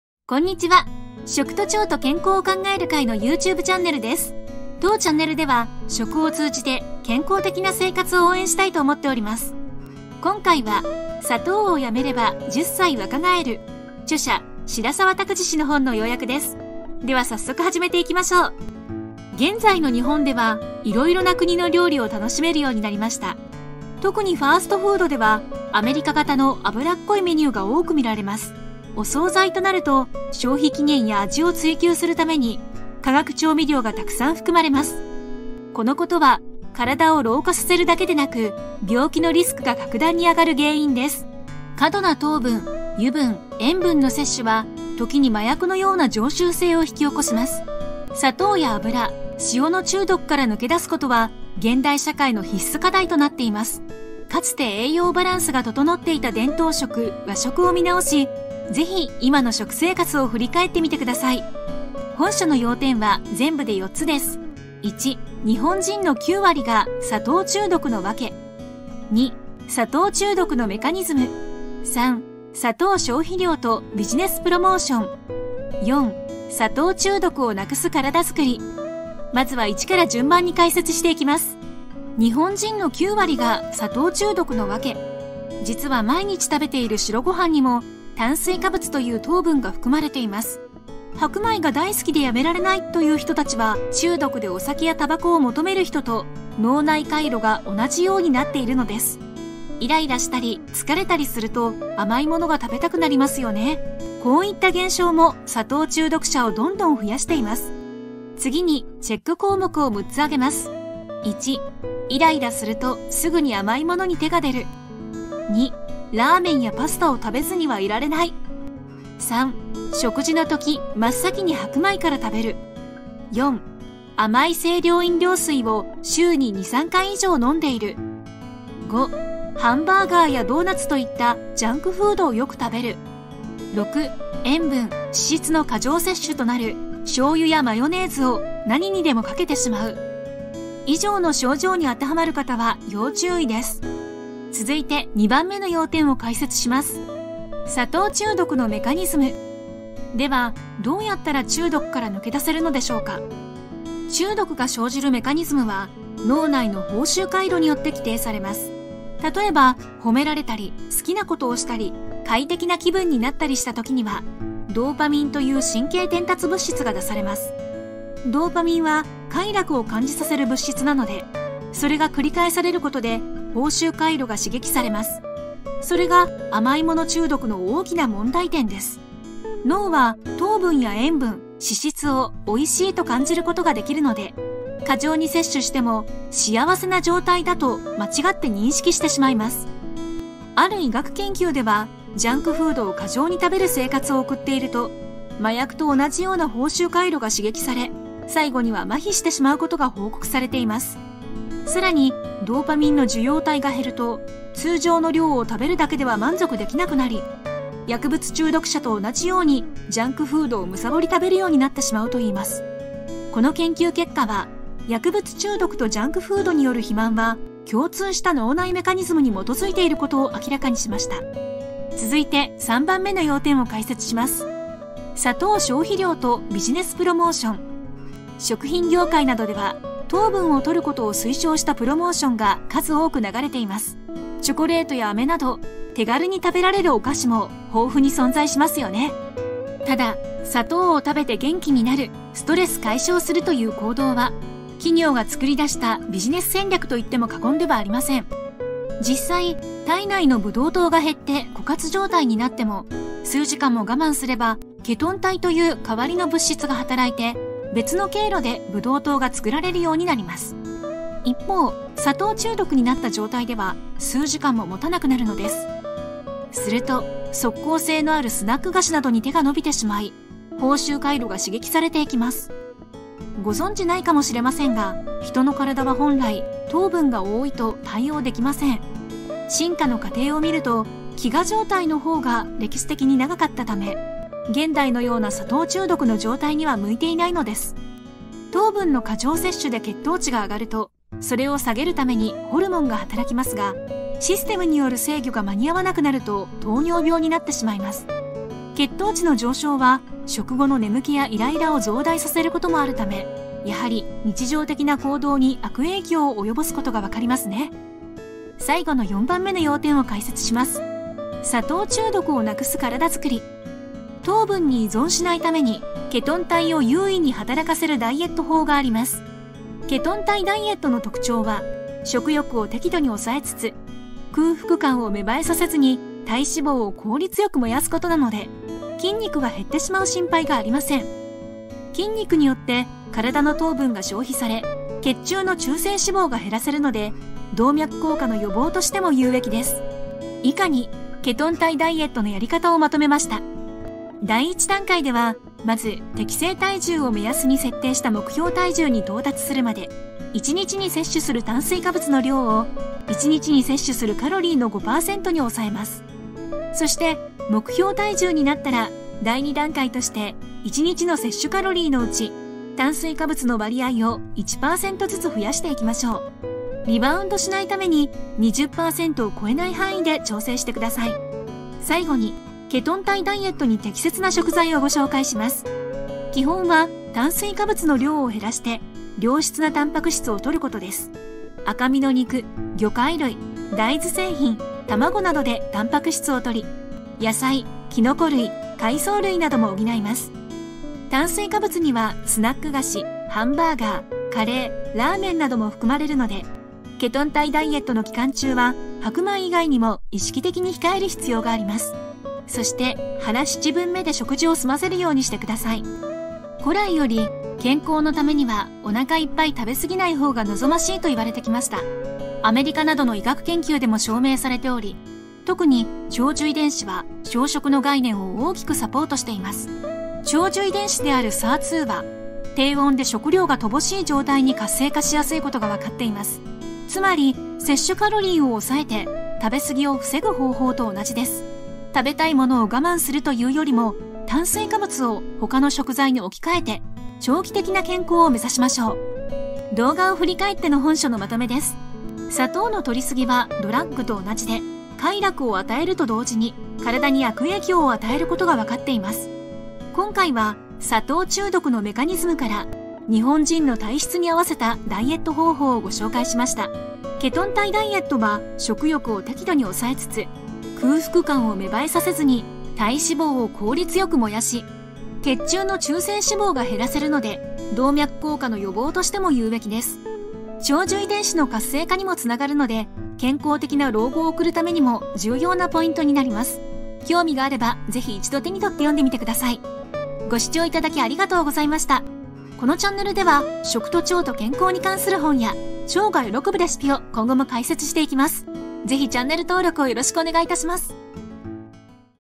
【本要約】「砂糖」をやめれば10歳若返る【白澤卓二】
00:00   はじめに
01:23   日本人の9割が砂糖中毒のわけ
02:28   砂糖中毒のメカニズム
03:44   砂糖消費量とビジネスプロモーション
05:11   砂糖中毒をなくす体づくり
07:21   まとめ

過度な糖分、油分、塩分の摂取は、ときに麻薬のような常習性を引き起こします。砂糖や油、塩の中毒から抜け出すことは、現代社会の必須課題となっています。

本書の要点は全部で4つです。
1． 日本人の9割が砂糖中毒のわけ
2． 砂糖中毒のメカニズム
3． 砂糖消費量とビジネスプロモーション
4． 砂糖中毒をなくす体づくり

砂糖の摂りすぎはドラッグと同じで、快楽を与えると同時に体に悪影響を与えることがわかっています。

本の著者：白澤卓二　氏
ぜひ【 食と腸の健康チャンネル 】登録をよろしくお願いいたします。

（一般社団食と腸と健康を考える会のブログ）

　ブログもお読みください。

★２倍速でも視聴できます。